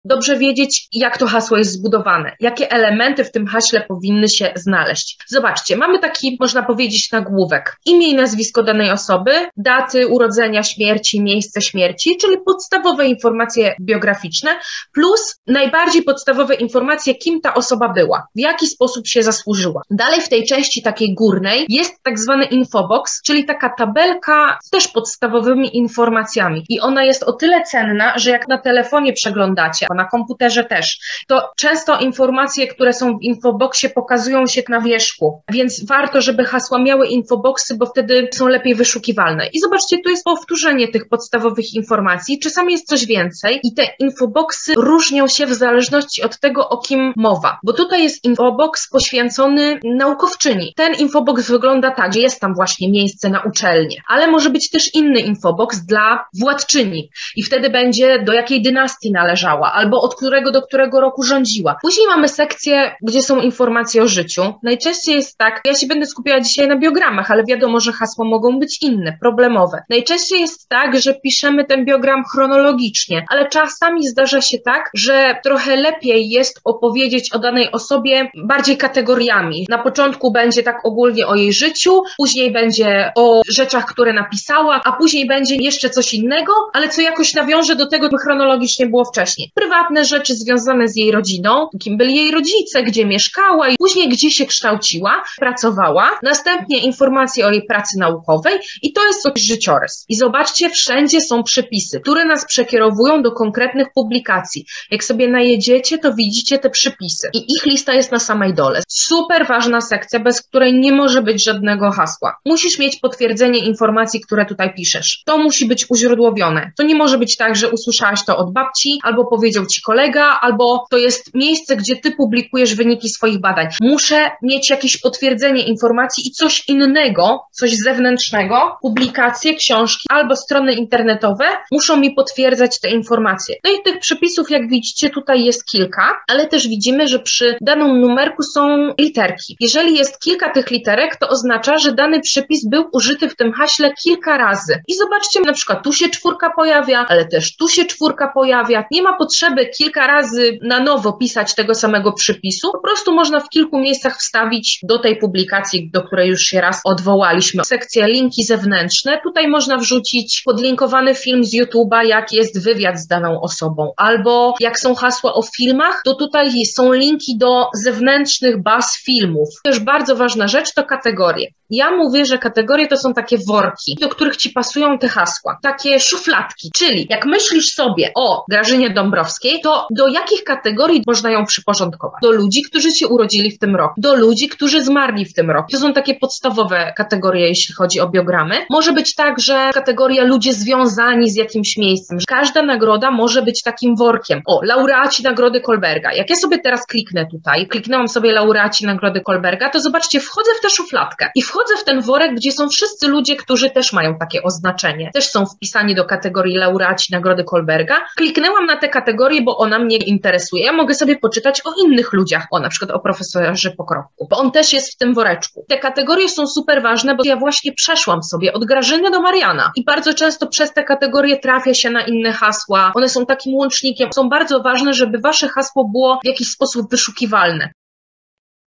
Ogólna budowa hasła w Wikipedii
Ogólna budowa hasła w Wikipedii. Fragment szkolenia Wikimedia Polska. Mówi Maria Weronika Kmoch.